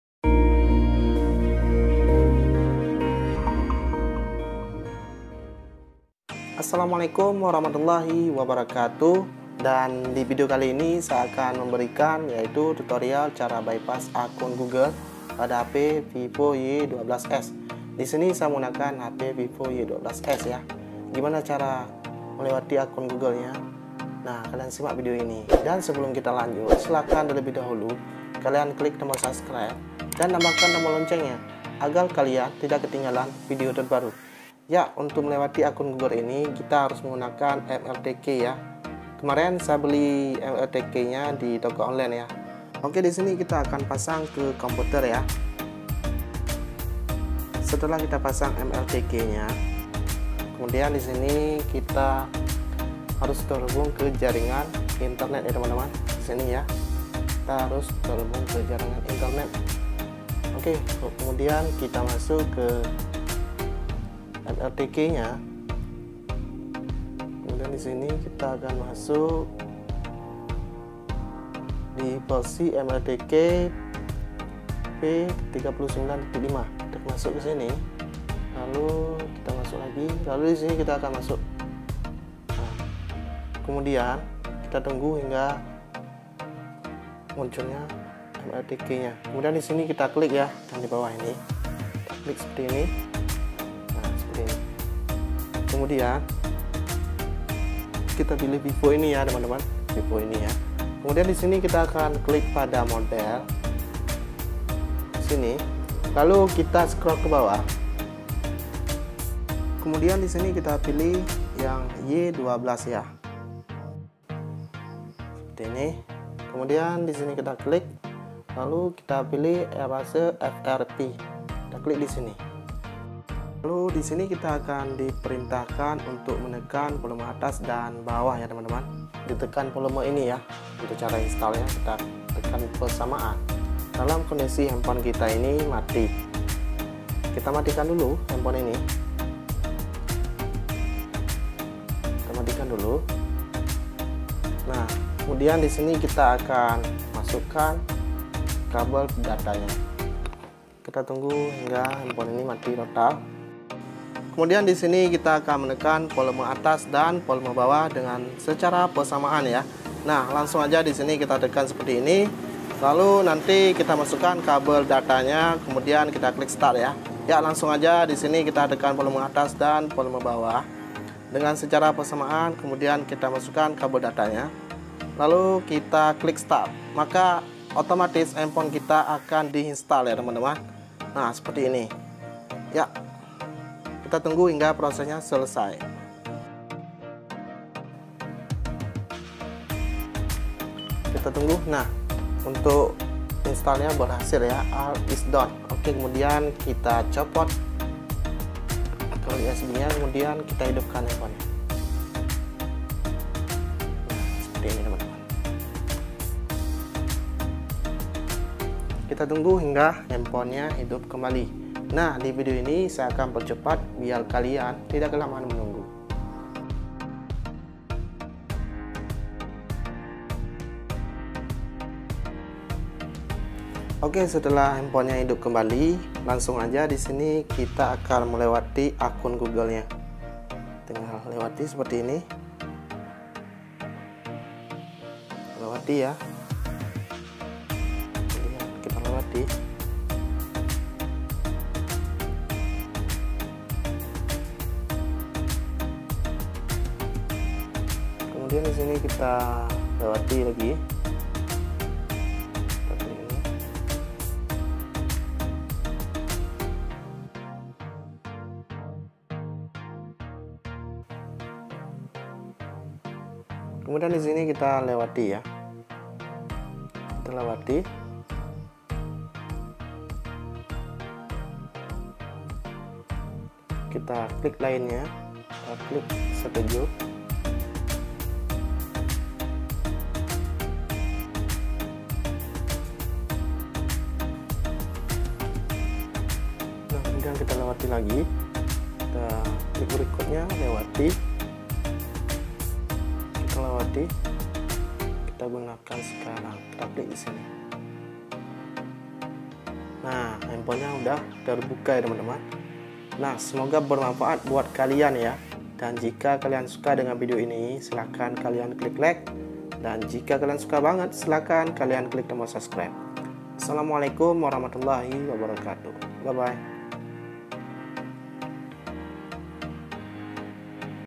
Cara bypass vivo y12s lupa akun google via mrt | frp vivo y12s via mrt
Terimakasih telah berkunjung ke channel saya kali ini saya akan berbagi tutorial cara bypass vivo y12s lupa akun google via mrt dan cara melewati akun google di hp vivo y12s untuk tutorial nya silahkan simak pada video di atas jangan sampai ada step-step ketinggalan pada video ini sekian dan terimakasih.